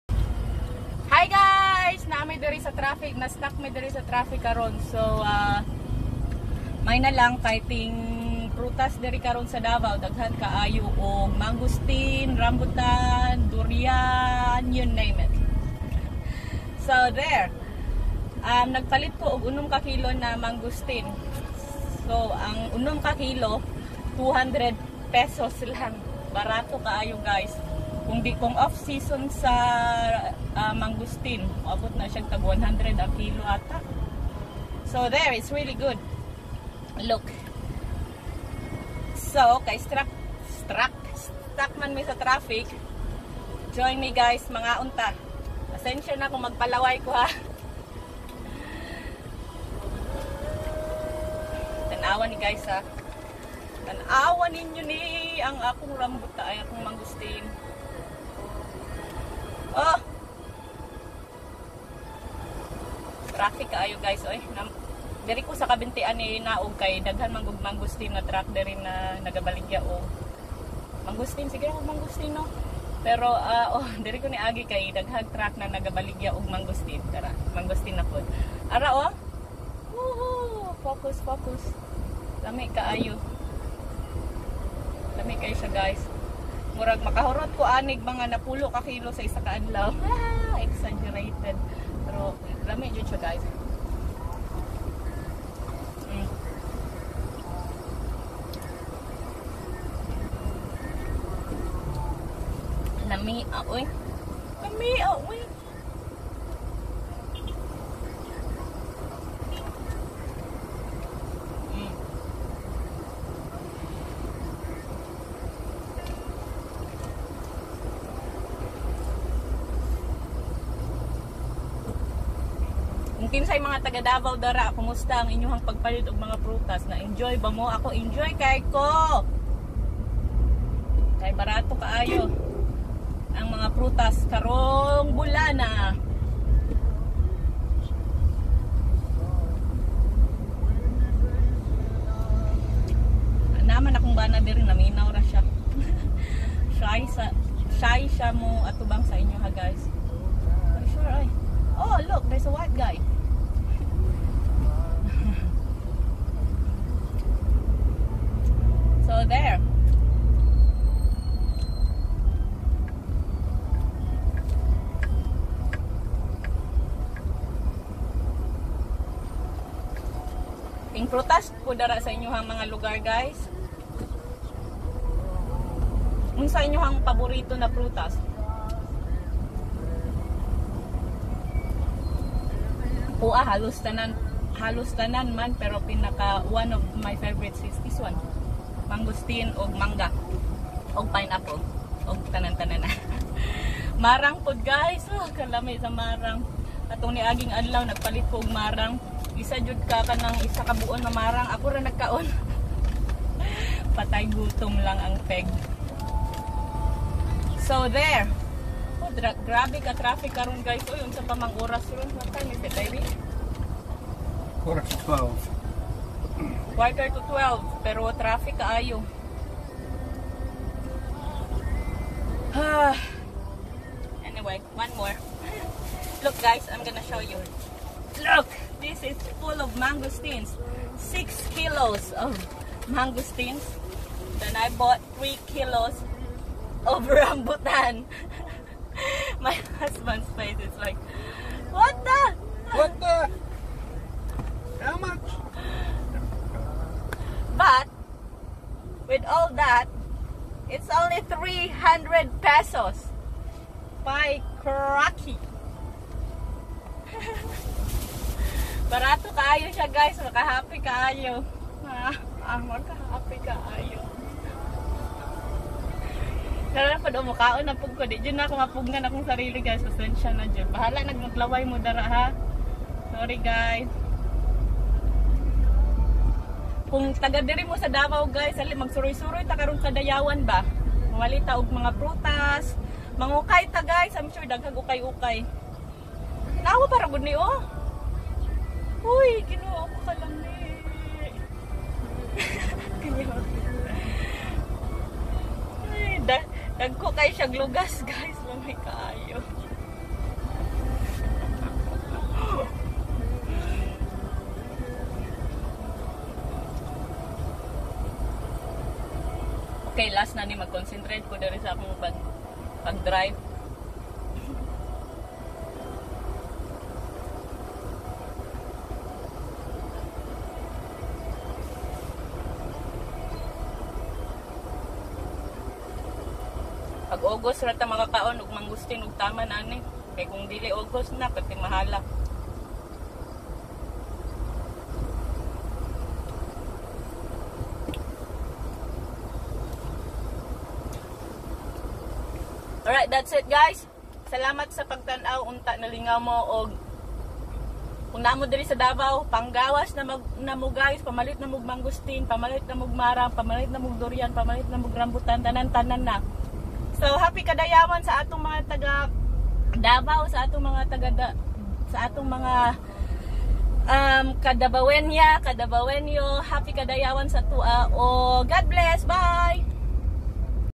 Bisdak, fruit season and Kadayawan in Davao city.
Just talking while stuck in traffic. Sorry if the camera is not level.